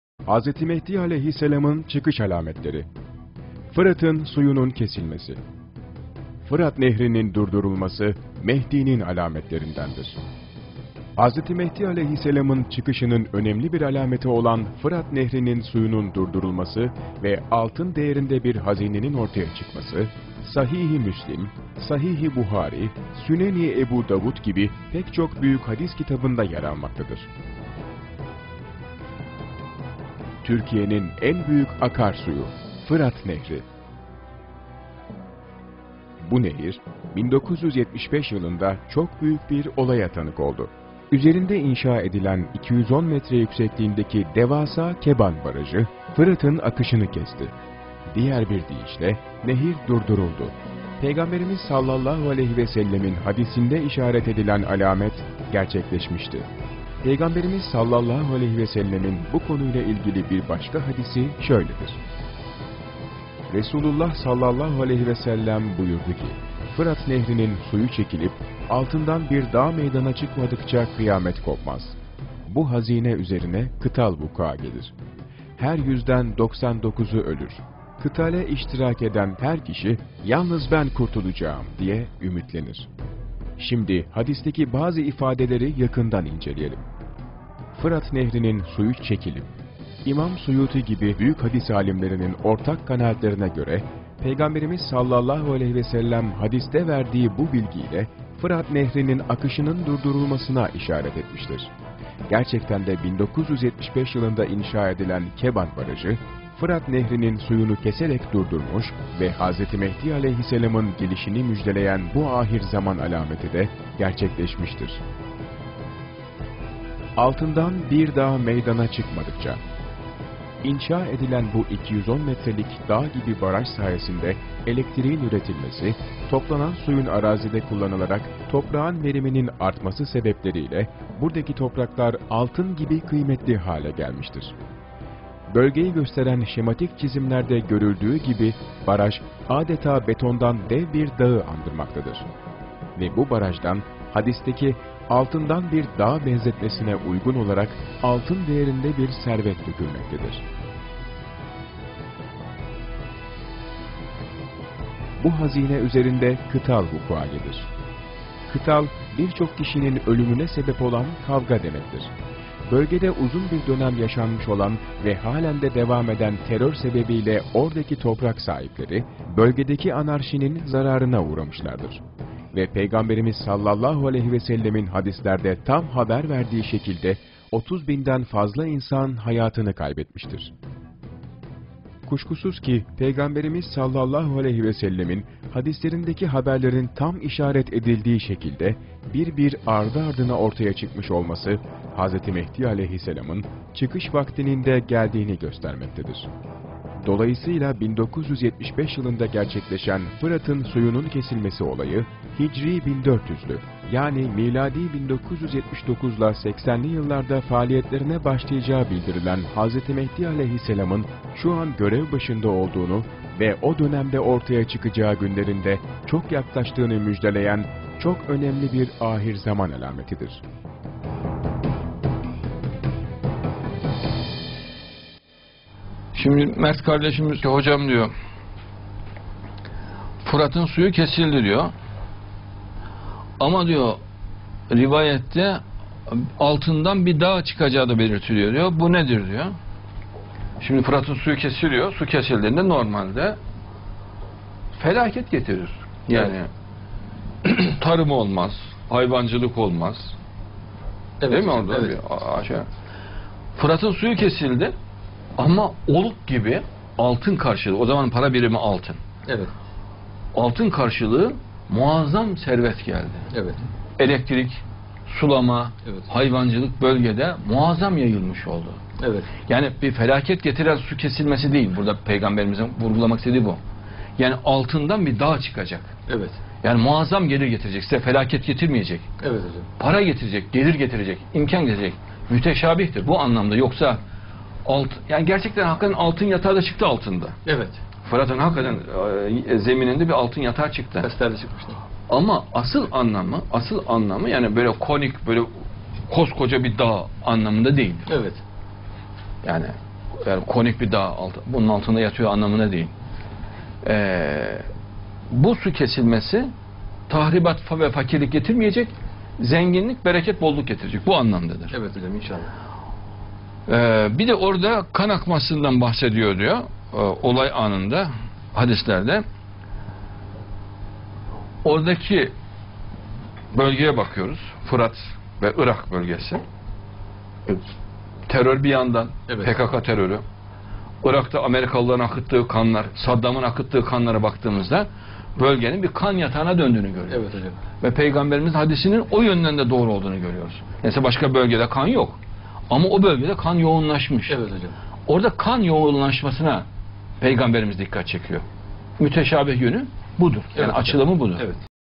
Hz. Mehdi (as)'ın çıkış alametleri: Fırat'ın suyunun kesilmesi
“Fırat nehrinin durdurulması Mehdi'nin alametlerindendir.”

(Kitab-ül Burhan Fi Alameti-il Mehdiyy-il Ahir Zaman, s. 39)

Hz. Mehdi (a.s.)'ın çıkışının önemli bir alameti olan ‘Fırat Nehrinin suyunun durdurulması ve altın değerinde bir hazinenin ortaya çıkması’ Sahih-i Müslim, Sahih-i Buhari, Sünen-i Ebu Davudgibi pek çok büyük hadis kitabında yer almaktadır.

Türkiye’nin en büyük akarsuyu:

Fırat Nehri...

Bu nehir 1975 yılında çok büyük bir olaya tanık oldu. Üzerinde inşa edilen 210 m. yüksekliğindeki devasa Keban Barajı, Fırat’ın akışını kesti, diğer bir deyişle nehir “durduruldu”. Peygamberimiz (sav)'in hadisinde işaret edilen alamet, gerçekleşmişti.

Peygamberimiz (sav)'in bu konuyla ilgili bir başka hadisi şöyledir:

Resulullah (sav) buyurdu ki: Fırat nehrinin suyu çekilip altından bir dağ meydana çıkmadıkça kıyamet kopmaz. Bu hazine üzerine kıtal vukua gelir, her yüzden doksan dokuzu ölür. Kıtale iştirak eden her kişi “yalnız ben kurtulacağım” diye ümitlenir. (Hadisi Buhari ve Müslim, Riyazü's Salihin, 3/332)

Şimdi hadisteki bazı ifadeleri yakından inceleyelim:

“Fırat nehrinin suyunun çekilip...”

İmam Suyuti gibi büyük hadis alimlerinin ortak kanaatlerine göre Peygamberimiz (sav), hadiste verdiği bu bilgi ile, ‘Fırat Nehri’nin akışının durdurulması’na işaret etmiştir. Gerçekten de 1975 yılında inşa edilen Keban Barajı, Fırat Nehri’nin suyunu keserek durdurmuş ve Hz. Mehdi (a.s.)'ın gelişini müjdeleyen bu Ahir Zaman alameti de gerçekleşmiştir.

"Altın"dan bir dağ meydana çıkmadıkça..."

İnşa edilen bu 210 metrelik “dağ” gibi baraj sayesinde elektriğin üretilmesi, toplanan suyun arazide kullanılarak toprağın veriminin artması sebeplerle, buradaki topraklar "altın" gibi kıymetli hale gelmiştir.

Bölgeyi gösteren şematik çizimlerde görüldüğü gibi baraj, adeta betondan dev bir dağı andırmaktadır. Ve bu barajdan, hadisteki ‘altından bir dağ’ benzetmesine uygun olarak, altın değerinde bir servet dökülmektedir.

“ Bu hazine üzerinde kıtal vukua gelir...”

‘Kıtal’, ‘bir çok kişinin ölümüne sebep olan kavga’ demektir. Bölgede uzun bir dönem yaşanmış olan ve halen de devam eden terör sebebi ile oradaki toprak sahipleri, bölgedeki anarşinin zararına uğramışlardır. Ve Peygamberimiz (sav)'in hadislerde tam haber verdiği şekilde, 30 binden fazla insan hayatın kaybetmiştir.

Kuşkusuz ki Peygamberimiz (sav)'in hadislerindeki haberlerin tam işaret edildiği şekilde, bir bir, ardı ardına ortaya çıkmış olması, Hz. Mehdi (a.s.)’ın çıkış vaktinin de geldiğini göstermektedir.

Dolayısıyla 1975 yılında gerçekleşen Fırat’ın suyunun kesilmesi olayı, Hicri 1400’lü yani Miladi 1979-80’li yıllarda faaliyetlerine başlayacağı bildirilen Hz. Mehdi (a.s.)'ın şu an görev başında olduğunu ve o dömende ortaya çıkacağı günlerin de çok yaklaştığını müjdeleyen çok önemli bir ahir zaman alametidir.